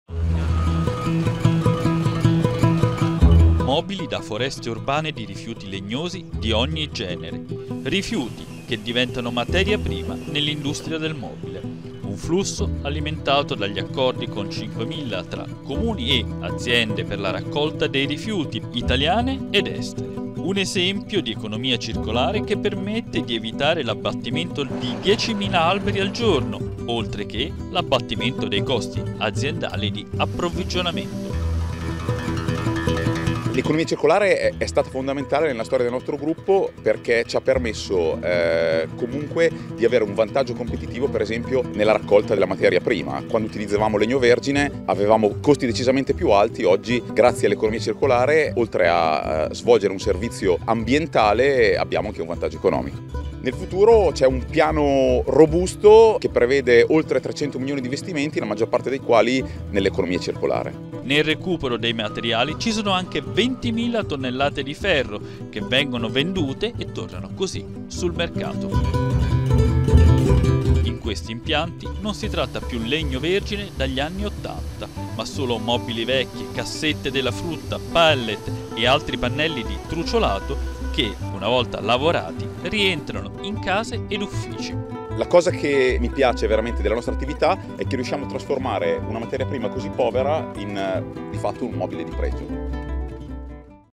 GRUPPO SAVIOLA - Intervista al Presidente Alessandro Saviola (Rai News 24)
Il servizio di Rai News 24 racconta il modello sostenibile di GruppoSaviola, rispettoso dell’ambiente e in grado di creare un vero vantaggio competitivo economico ed ecologico.
Ai microfoni di Matteo Parlato, il Presidente Alessandro Saviola annuncia il piano di investimenti green in atto basato sull'Economia Circolare.